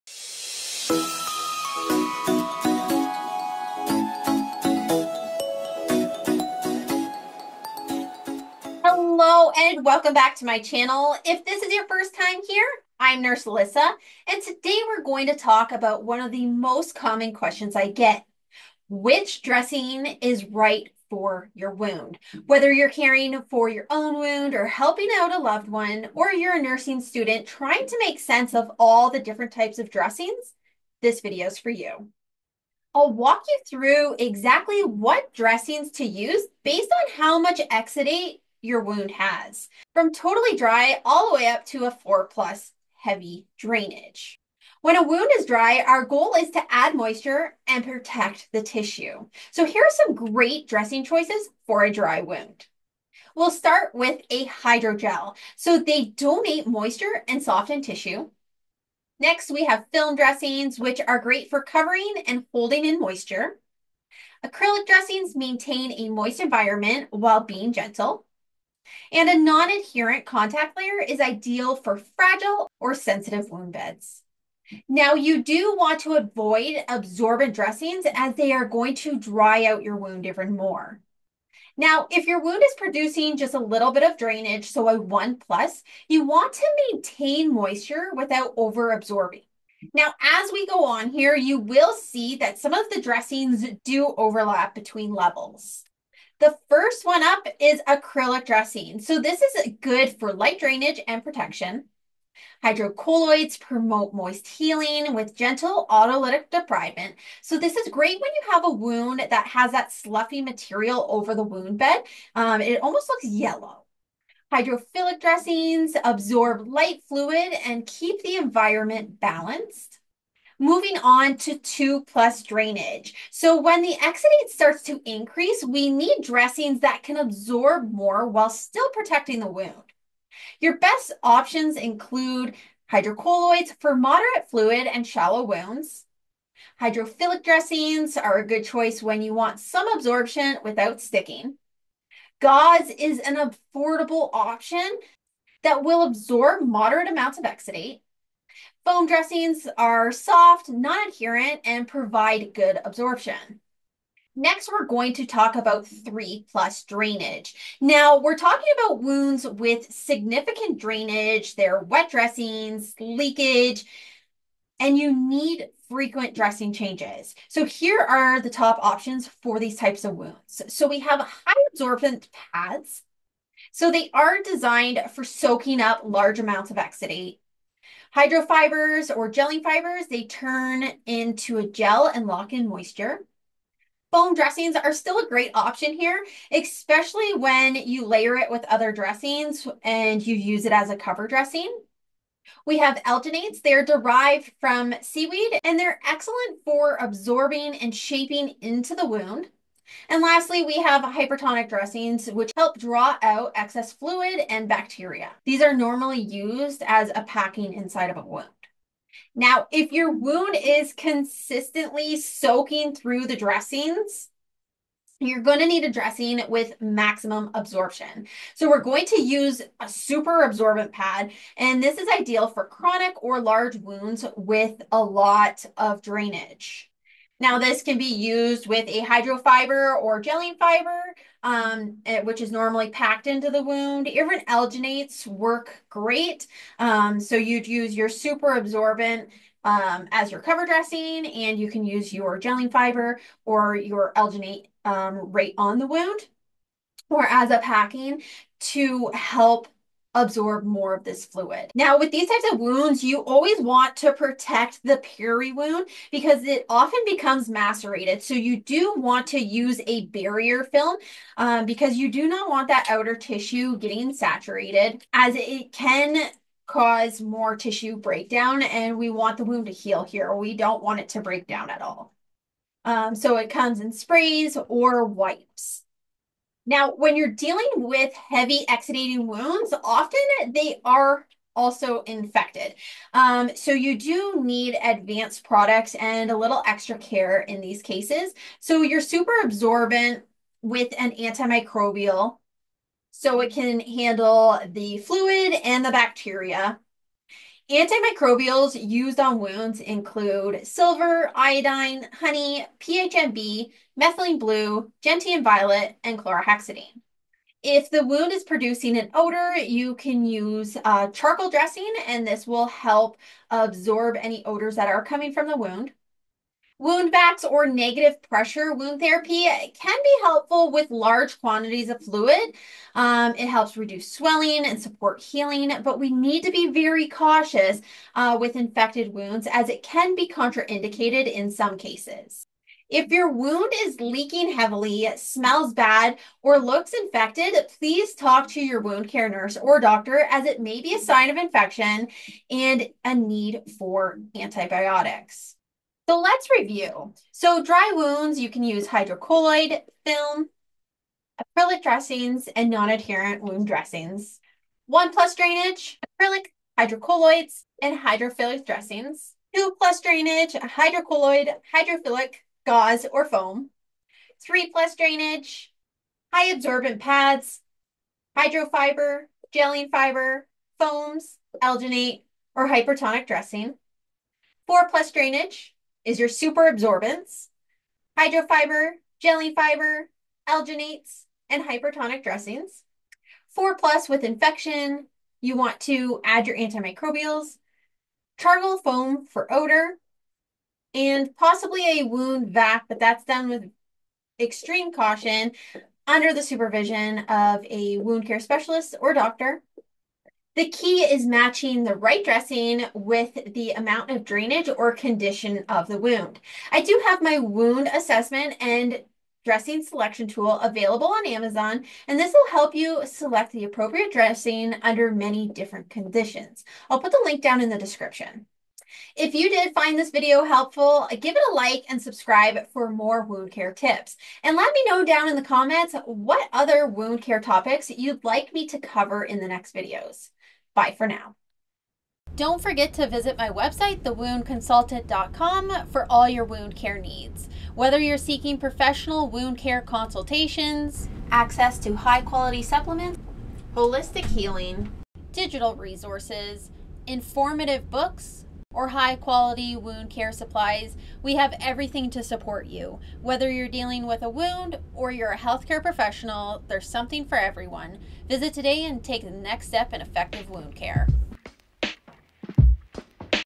Which Dressing Is Right for Your Wound? | Exudate Level
📍 Which wound dressing should you use? In this video, Nurse Alyssa breaks it down by exudate level—that’s how much fluid your wound is producing. From completely dry wounds to heavily draining or infected wounds, you’ll learn which dressing is best for your wound’s specific needs.

👩‍⚕️ You’ll learn about:

Dressing types like hydrogel, foam, alginate, hydrocolloid, and superabsorbents

How to assess your wound’s exudate level (0 to 4+)

Special considerations for infected wounds

Why matching the dressing to drainage is essential for healing

This guide is perfect for patients, caregivers, and student nurses looking to improve their wound care knowledge.

✨Wound Assessment & Dressing Selection Tool

For all your wound care needs visit!
➤Wound Consultations
➤Supplements for wound healing
➤Holistic Healing
➤Digital recourses
➤Books